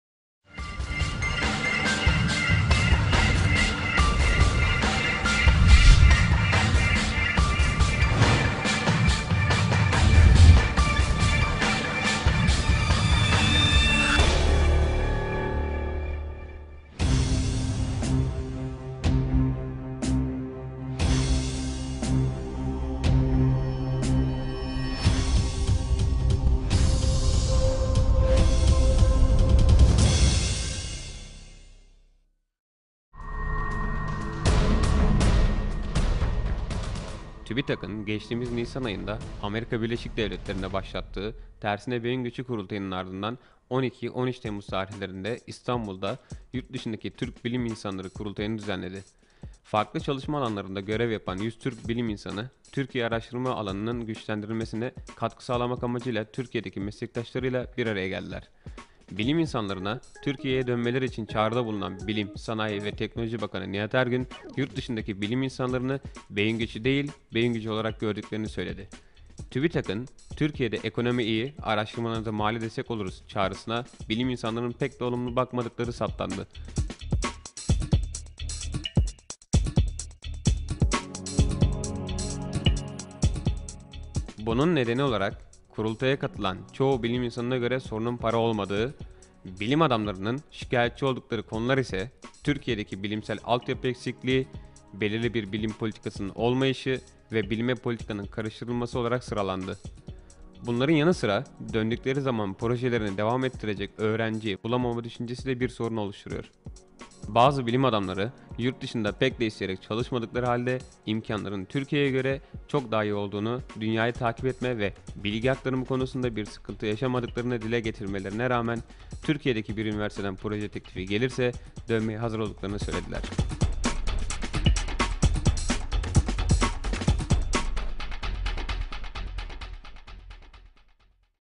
TÜBİTAK "Tersine Beyin Göçü"
Kadir Has Üniversitesi Pusula Akademisi'nin hazırlamış olduğu "Tübitak Tersine Beyin Göçü" haberi.